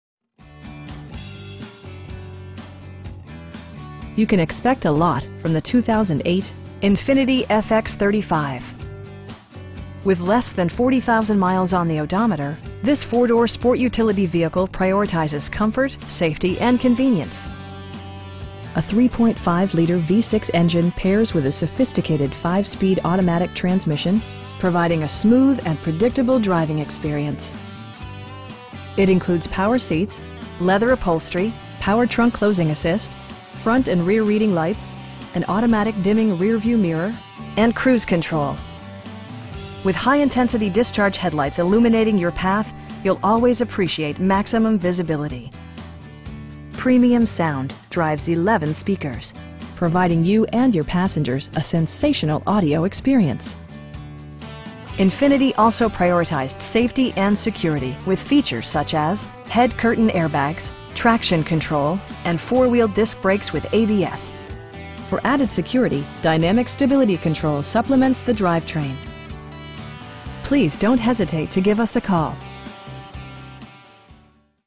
2008 Infiniti FX35 FX in Tracy, CA 95304
Tracy Honda

You can expect a lot from the 2008 Infiniti FX35. With less than 40,000 miles on the odometer, this 4 door sport utility vehicle prioritizes comfort, safety and convenience. A 3.5 liter V-6 engine pairs with a sophisticated 5 speed automatic transmission, providing a smooth and predictable driving experience. It includes power seats, leather upholstery, power trunk closing assist, front and rear reading lights, a trip computer, an automatic dimming rear-view mirror, remote keyless entry, and cruise control. With high intensity discharge headlights illuminating your path, you'll always appreciate maximum visibility. Premium sound drives 11 speakers, providing you and your passengers a sensational audio experience. Infiniti also prioritized safety and security with features such as: head curtain airbags, front SIDE impact airbags, traction control, brake assist, anti-whiplash front head restraints, a security system, and 4 wheel disc brakes with ABS. For added security, dynamic Stability Control supplements the drivetrain. A Carfax history report provides you peace of mind by detailing information related to past owners and service records. Our aim is to provide our customers with the best prices and service at all times. Please don't hesitate to give us a call.